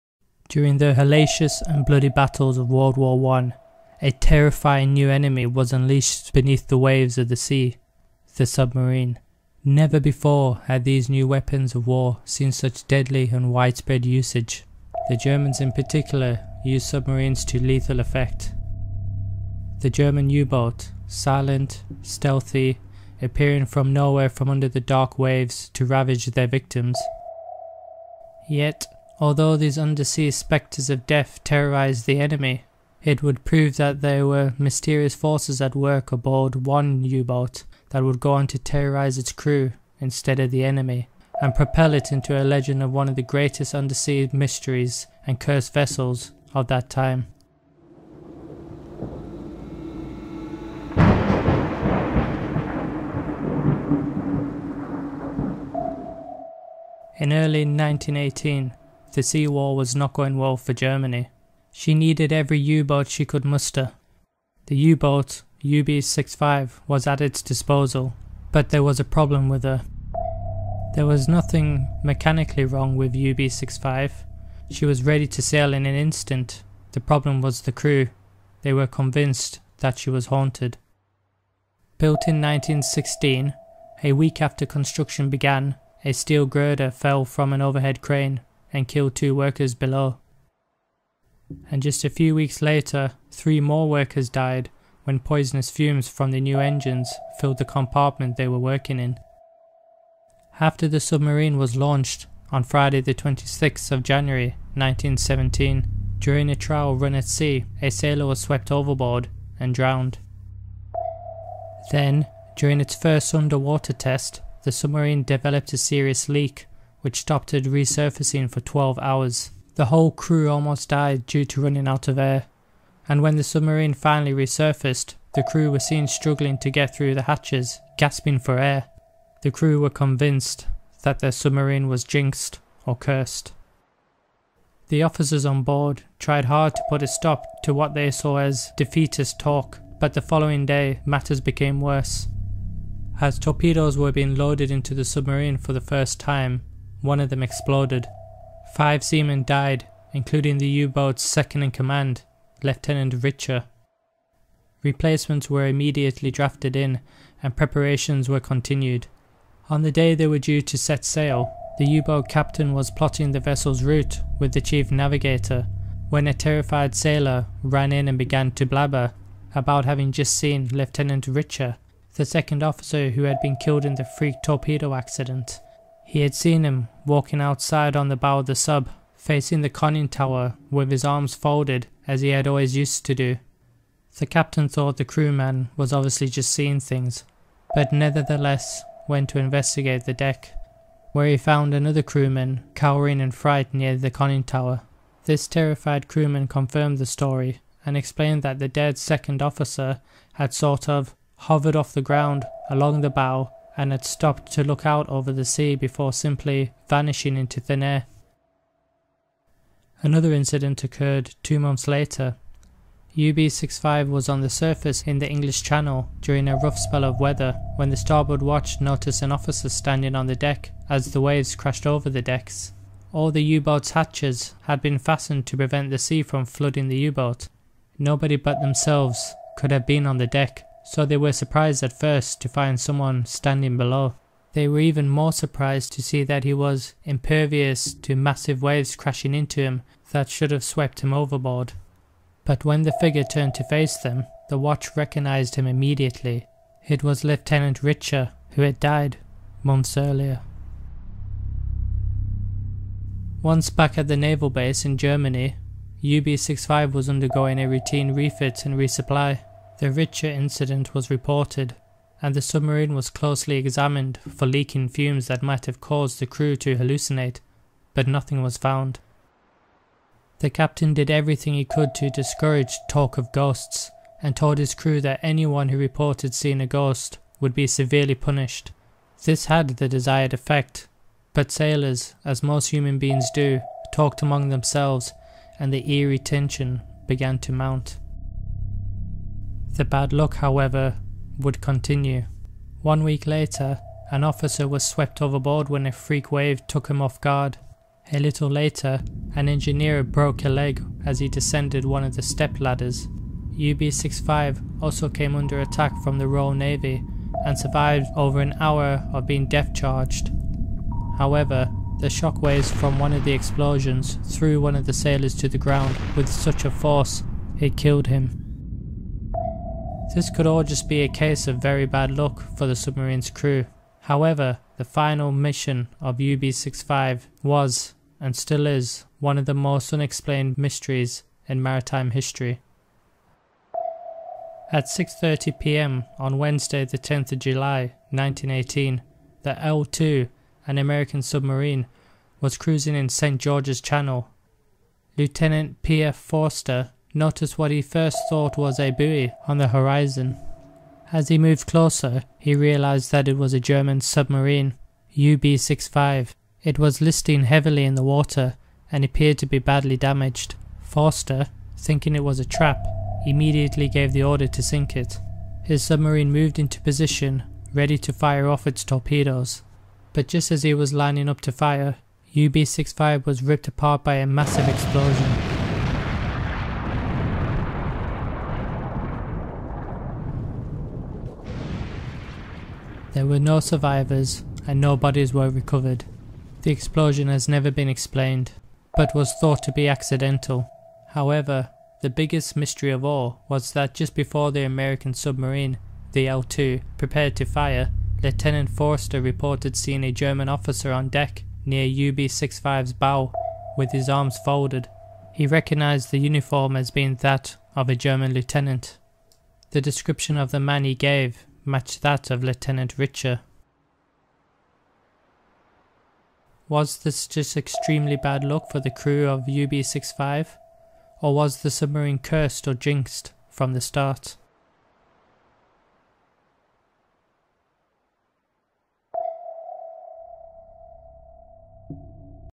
The Curse of UB-65 - The Haunted Submarine
During the bloody battles of World War 1, a terrifying new enemy was unleashed beneath the waves of the sea; the submarine.

Never before had these new weapons of war seen such deadly and widespread usage. The Germans in particular put submarines to spectacularly lethal effect. The German U-Boat - Silent, stealthy, appearing from nowhere from under the dark waves to ravage their victims.

Yet although these undersea spectres of death terrorised the enemy, it would prove that there were mysterious forces at work aboard one U-boat that would go on to terrorise its crew instead of the enemy, and propel it into a legend of one of the greatest undersea mysteries and cursed vessels of that time.

In early 1918 the sea war was not going well for Germany. She needed every U-boat she could muster. The UB-65 was at its disposal, but there was a problem with her. There was nothing mechanically wrong with UB-65, she was ready to sail. The problem was the crew. They were convinced that she was haunted...